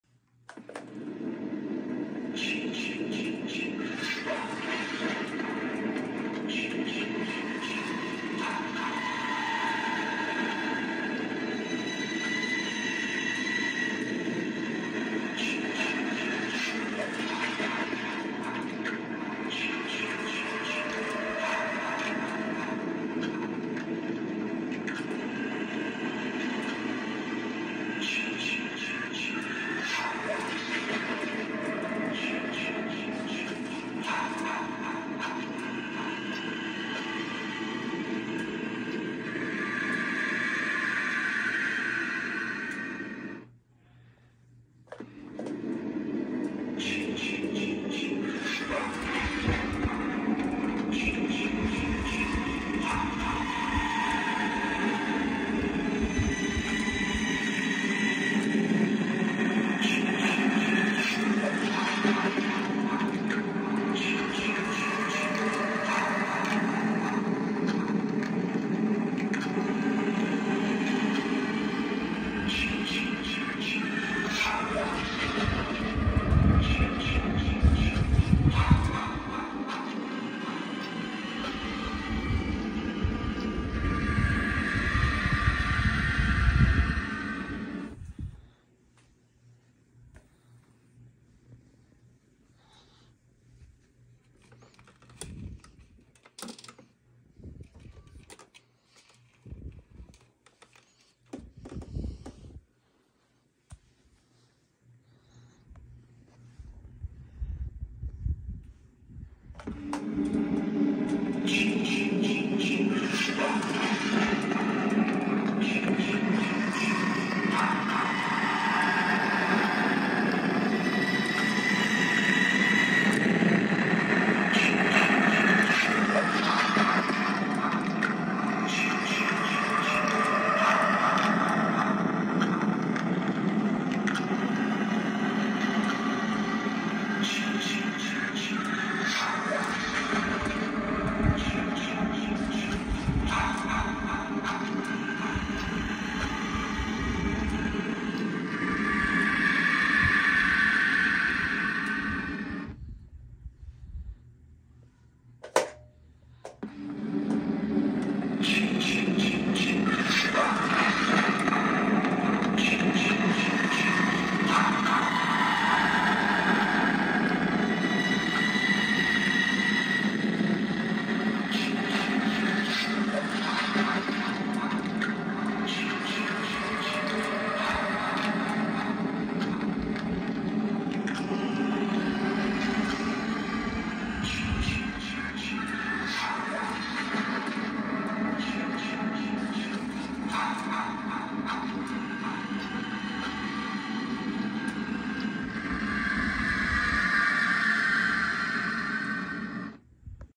Kmart Exclusive Gemmy’s 2010 Jason Voorhees 6.4ft LifeSize Halloween Animatronic Friday the 13th
I originally bought this 2010 Jason from Mercari for like $400 total and the seller had stated that he had some minor issues like no body turning, missing his original machete and has a crack on his plastic arm that moves. Upon receiving him and turning him on he basically did nothing except move his one eye and play his sound, that’s it. I was luckily able to fix his body turn and head turn, all they needed was a new 10 tooth gear for each. I also attempted to fix his moving arm but sadly I don’t have a specific 10 tooth gear that he uses so as of now, I just cut a wire from his arm motor so it doesn’t cause too much stress in his motor by just sitting there trying to move. Eventually whenever I have the chance in finding that specific gear, his arm will be back to working order